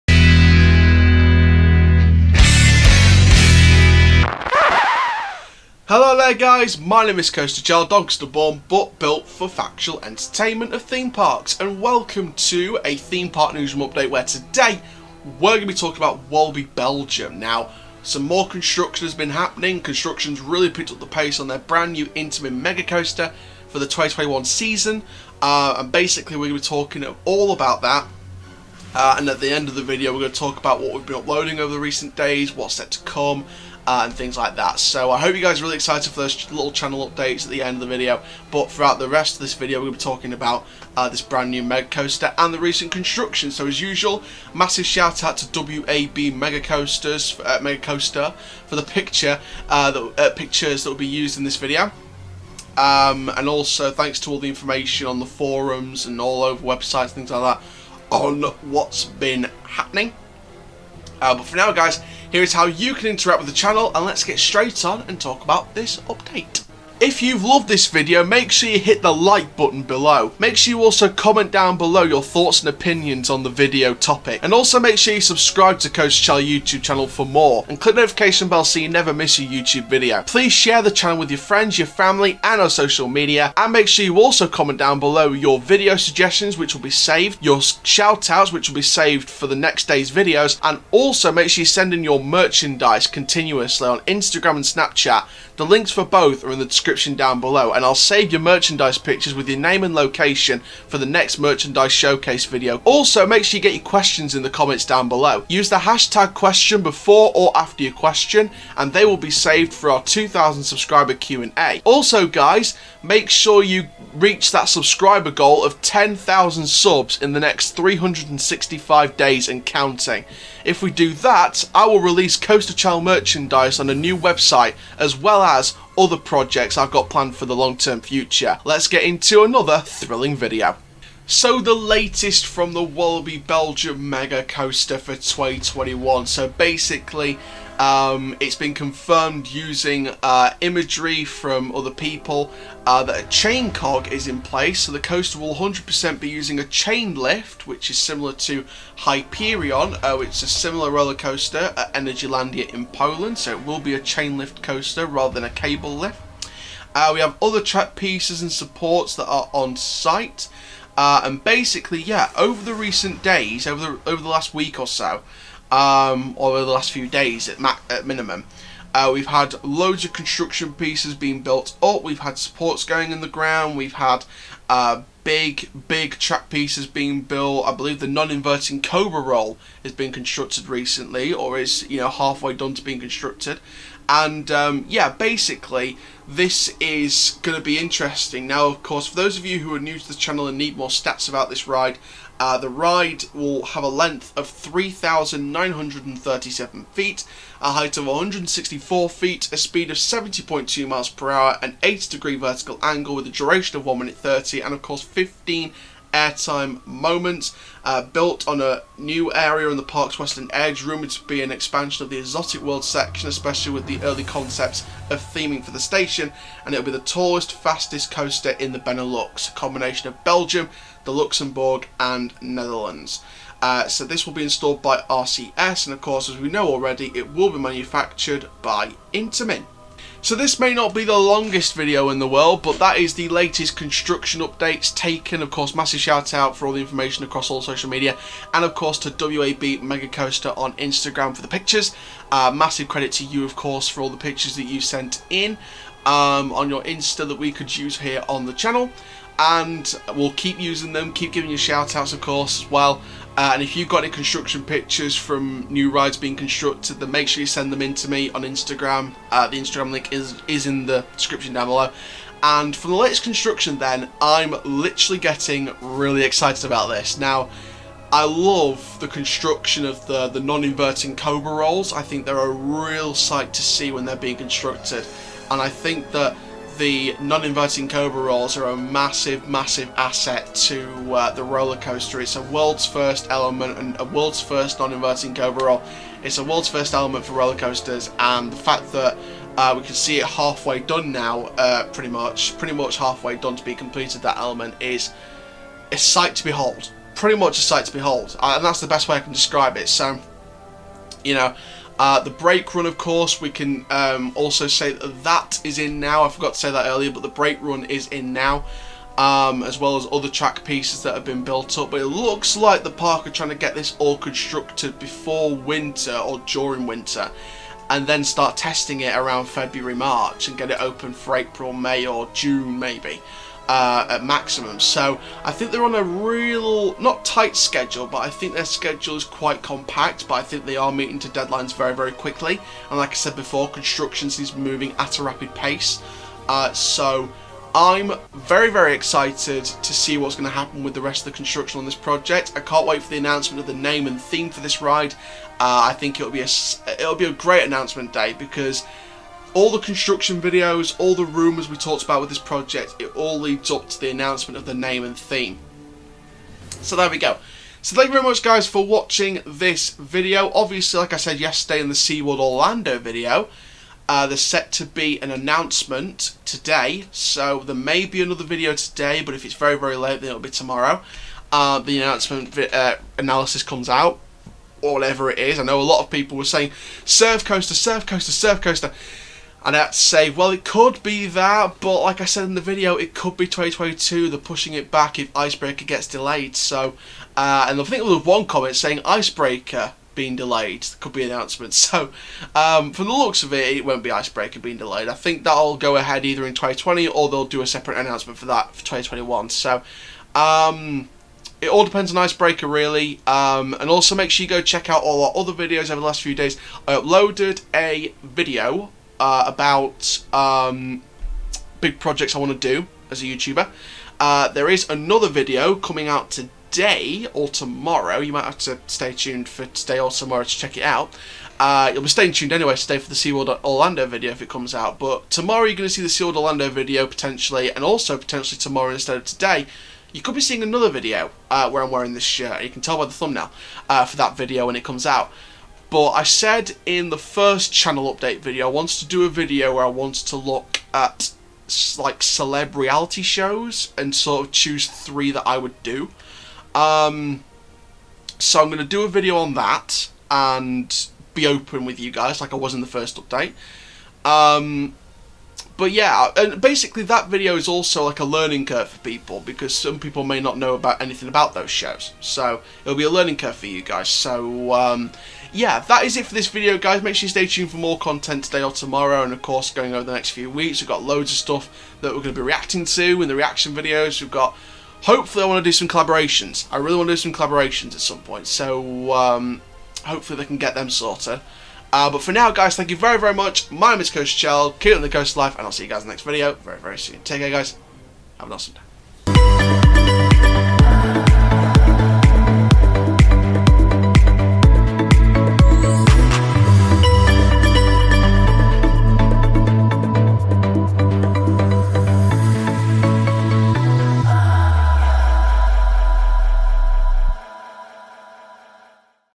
More Track Pieces Are In Place For the New Intamin Mega Coaster Opening In 2021 At Walibi Belgium
On this episode of Theme Park Newsroom, I'll be sharing more construction updates for Walibi Belgium's brand new Intamin Mega Coaster for 2021, as more track pieces are installed onsite. I'll share recent pictures and also my thoughts on the latest construction progress.

Intro: 0:00
Stats and Facts: 2:11
My Thoughts: 3:50
Outro: 8:09